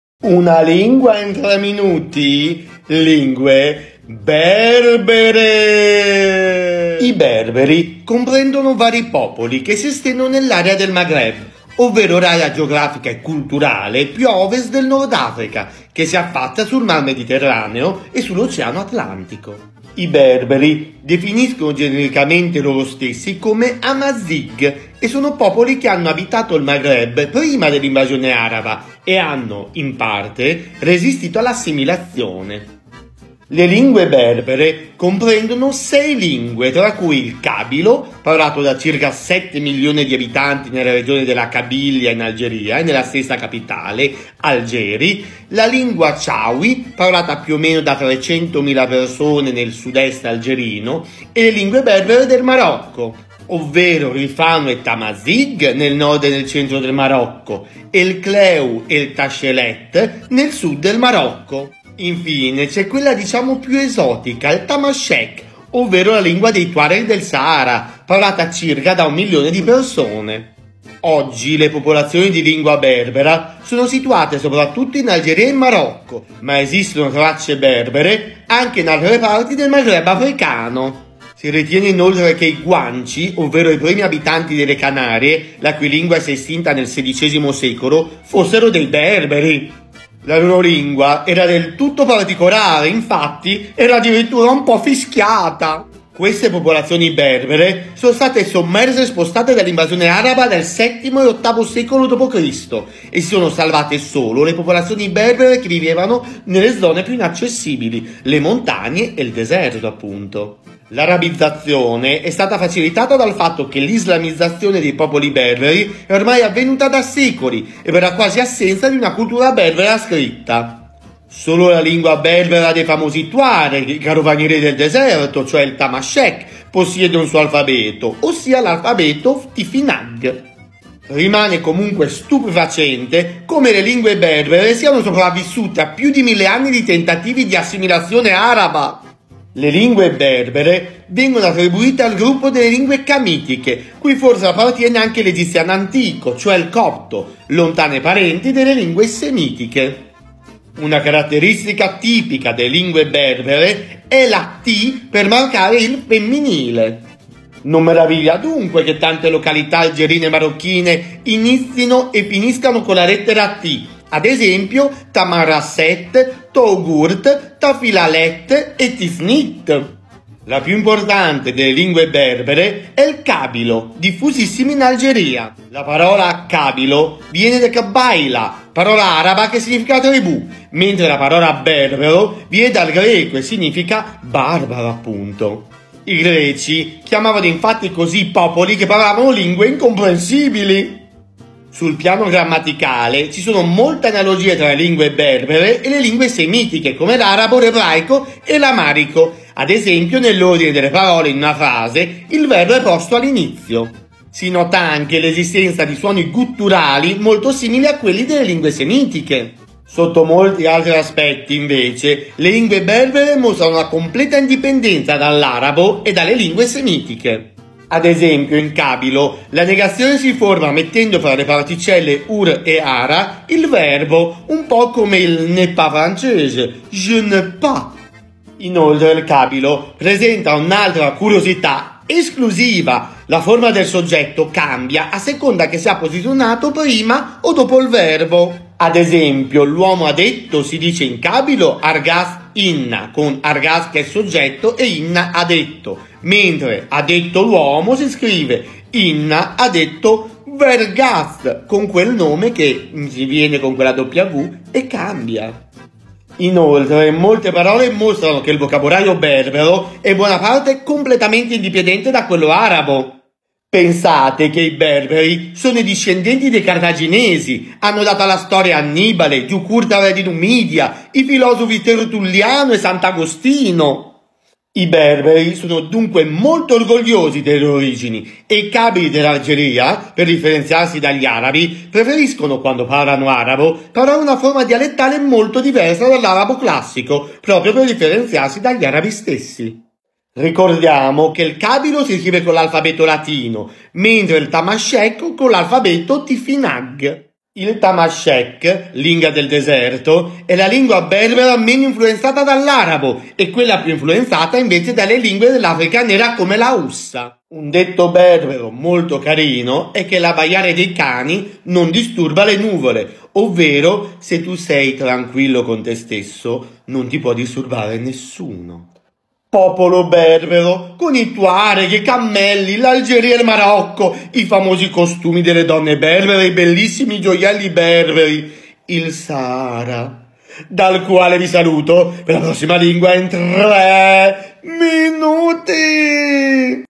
in 3 minuti!
Il berbero è la lingua dei Berberi; si tratta di una lingua camitica appartenente alla famiglia linguistica afro-asiatica, e quindi imparentata, tra l'altro, con l'egizio, ma anche con l'arabo e l'ebraico. Le fasi più antiche del berbero sono rappresentate dall'antico libico.

In italiano, "berbero" deriva dal francese berbère, che a sua volta riprende la pronuncia magrebina dell'arabo barbar. Il termine arabo probabilmente non fa che continuare il latino barbarus, con cui venivano chiamate, nell'Impero romano, le popolazioni di lingua non latina[1]. Un'etimologia popolare, riportata già da Ibn Khaldūn, afferma che gli Arabi, non capendo chi parlava il berbero, dicevano che "emetteva suoni inintelligibili" ("emettere suoni inintelligibili" in arabo si dice barbar) analogo alla parola in greco antico: βάρβαρος, bárbaros.

Oggi i berberi non amano più sentirsi chiamare con questo nome (in arabo barbar vuole dire anche "barbaro"), e per definirsi preferiscono usare il termine berbero amaziɣ (al plurale imaziɣen, "uomini liberi"), e per la lingua berbera il corrispettivo femminile tamaziɣt.